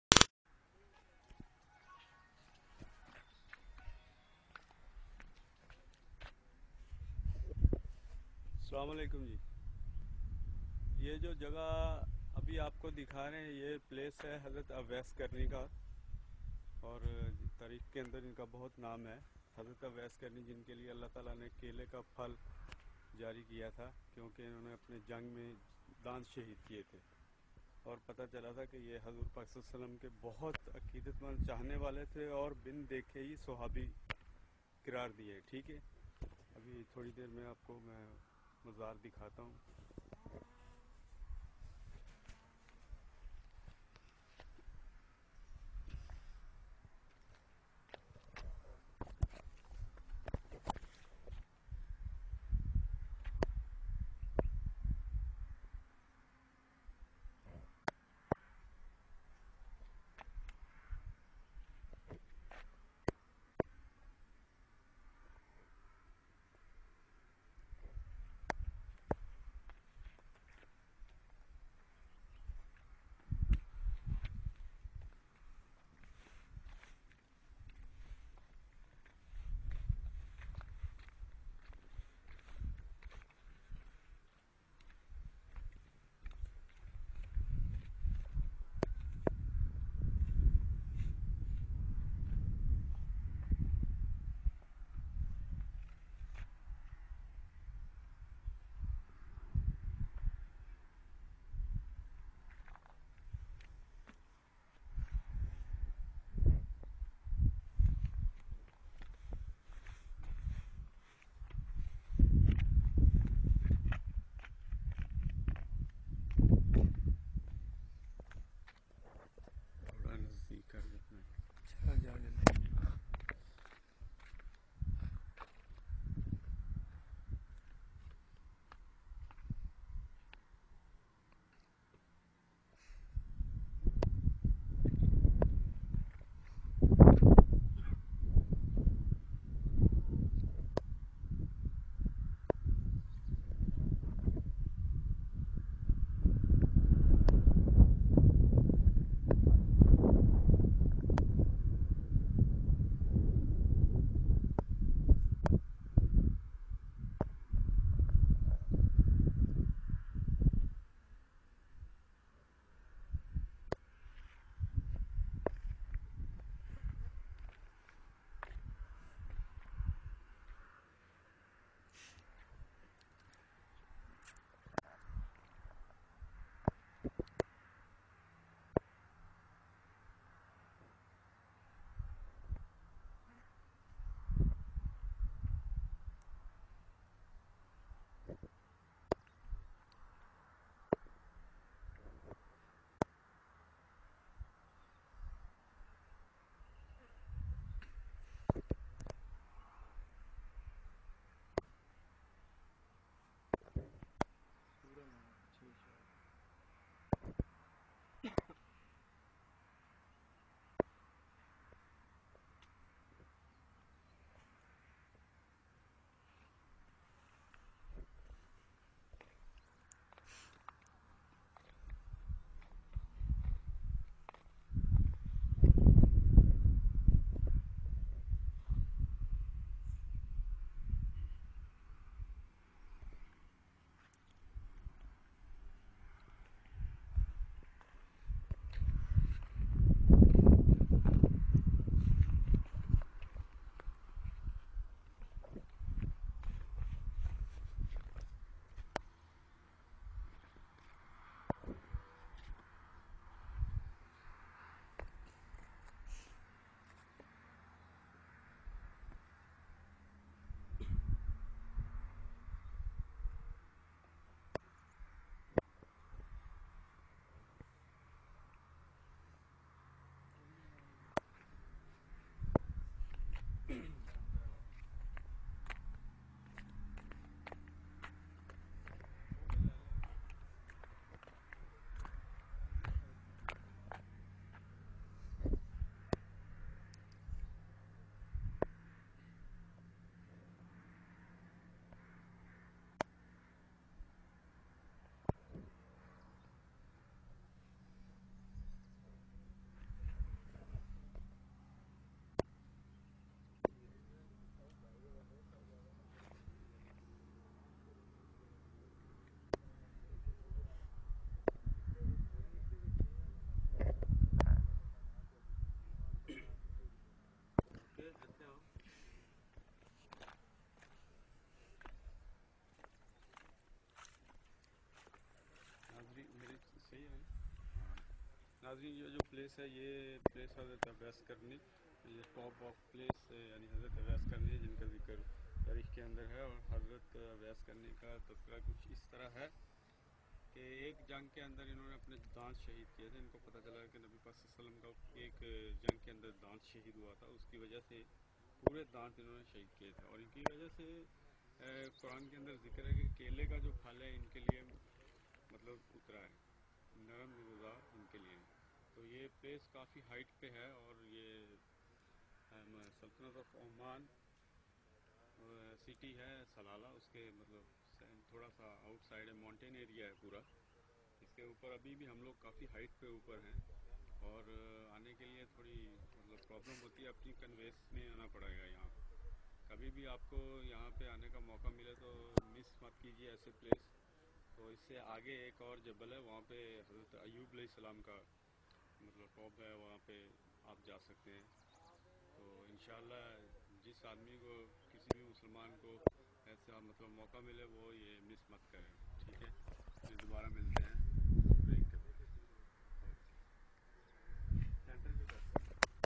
Tomb Of Hazrat Owais Qarni R.A
Uwais al-Qarani Arabic: also known as Saint Uwais al-Qarani, was an Arab, Muslim, mystic, martyr, and philosopher from Yemen. He lived during the lifetime of Prophet Muhammad, but never met Prophet Muhammad personally.